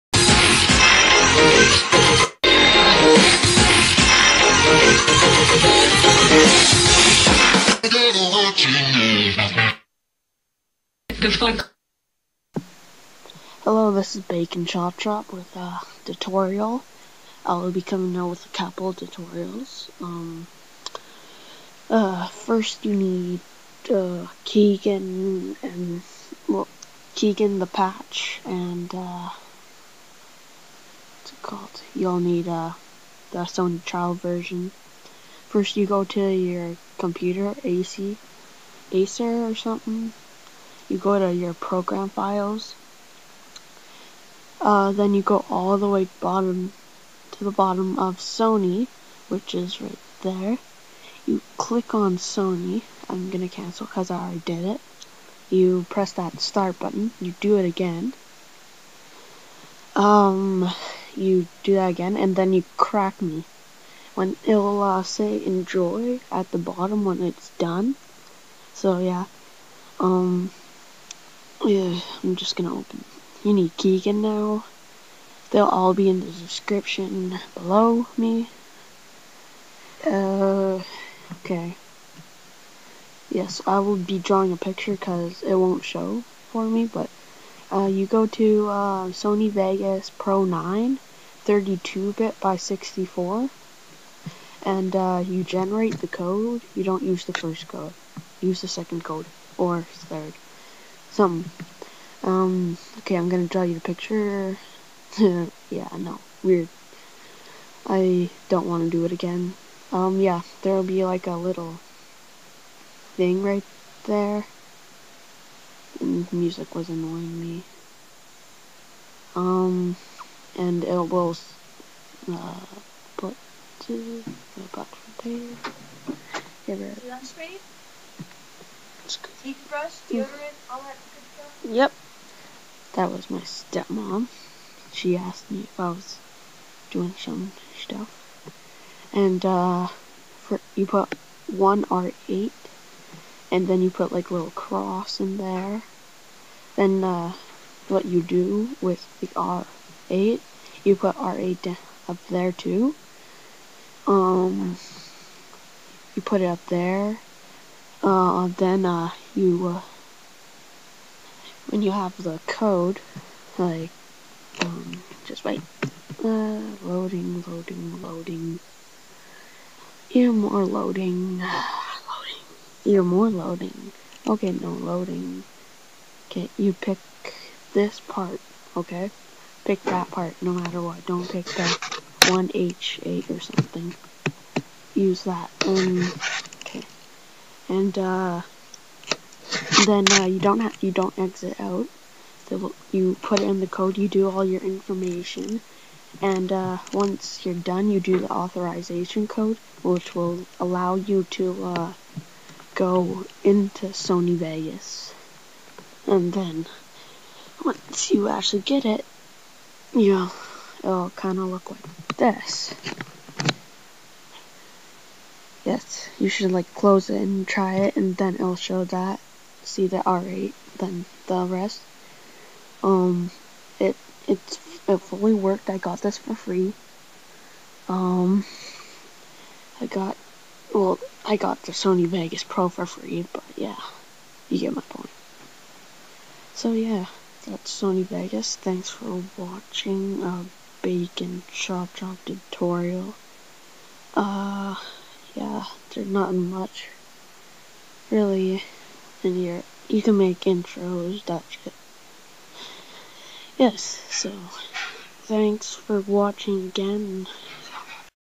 sony vegas pro 10 full free version
How to download Sony Vegas Pro 10 FULL version for Free!